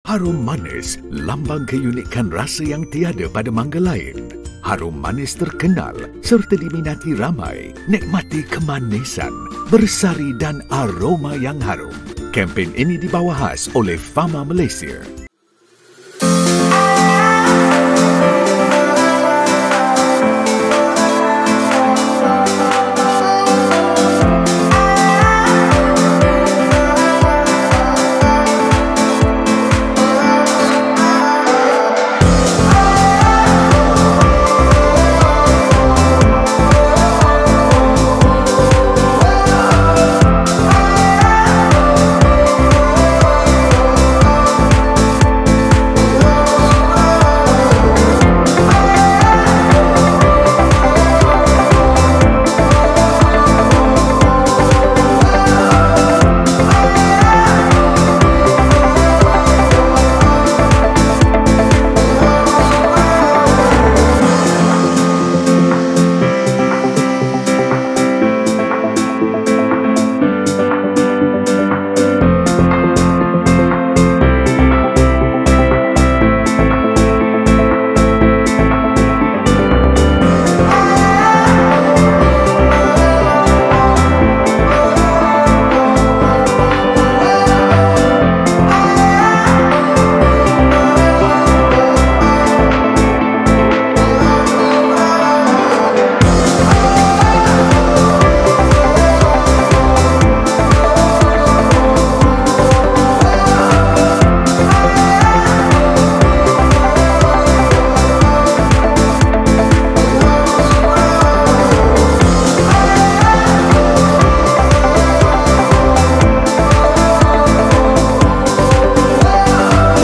Vlog Pelancaran Harum Manis Challenge
Pelancaran Harum Manis Challenge
oleh Yb Tuan Sim Tze Tzin
Timbalan Menteri Pertanian dan Industri Asas Tani